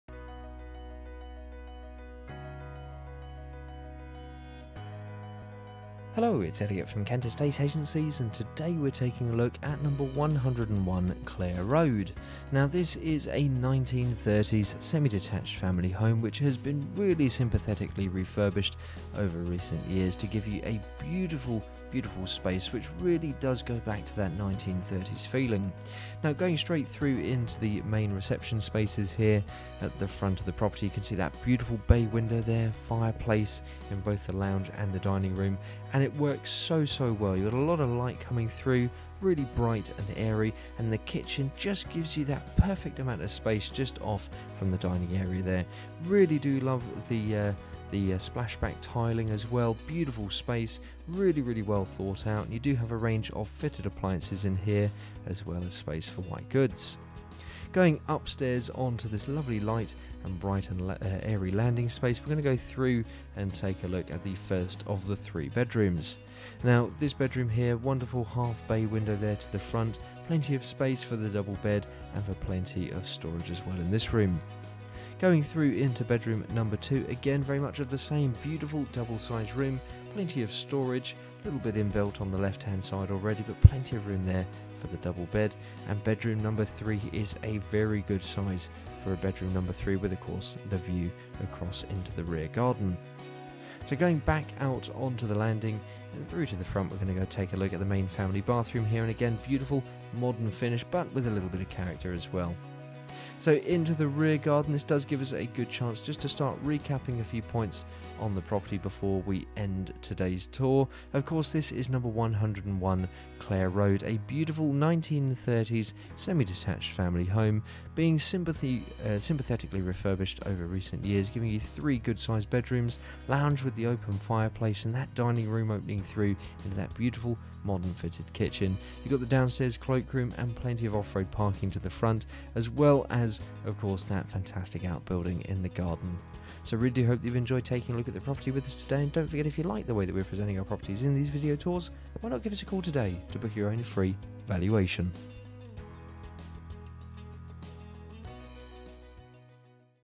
Clare Road Narrated Video
This light and airy home is perfectly situated for all the family in a popular central location within a short stroll of Whitstable mainline railway station and the well regarded St Mary's Primary School.
This charming 1930's semi-detached house benefits from excellent natural light and is well presented throughout.
The accommodation incorporates spacious entrance hall, downstairs cloakroom, lounge with bay window and open fireplace and dining room that opens to a fitted kitchen which gives direct access to rear and side gardens.  To the first floor are three good size bedrooms and a modern fitted family bathroom.
There is a 64ft southerly aspect side garden with a useful outbuilding and a courtyard styled rear garden.
Off road parking is provided to the front.  Regular bus services to surrounding towns and the Cathedral City of Canterbury (approx. 6.7 miles) are available about 175 yards.
Tankerton's delightful seafront and parade of shops, restaurants and cafes are just over half a mile.
The quaint town of Whitstable with its working harbour and array of individual shops and eateries is about 0.8 of a mile.
Call Kent Estate Agencies on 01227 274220 to book your viewing appointment